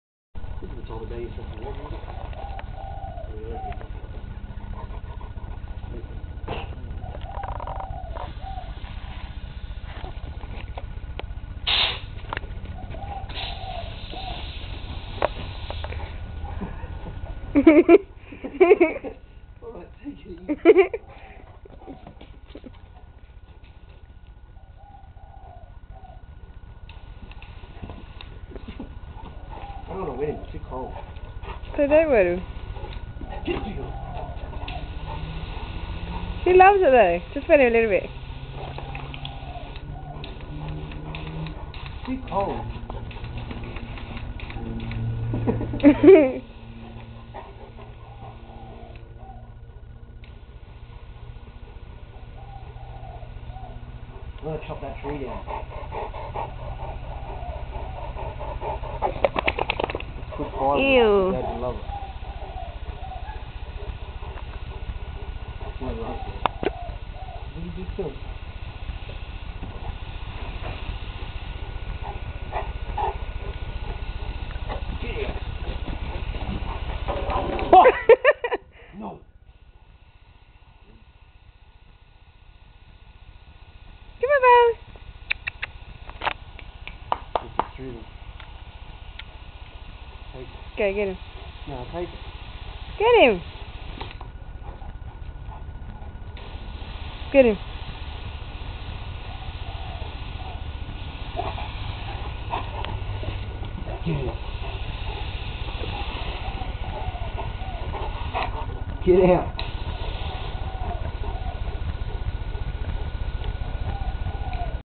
Neapolotan Mastiff Loves The Water!!
This our our dog Boss! He loves the water and goes a little wacko when we spray him!!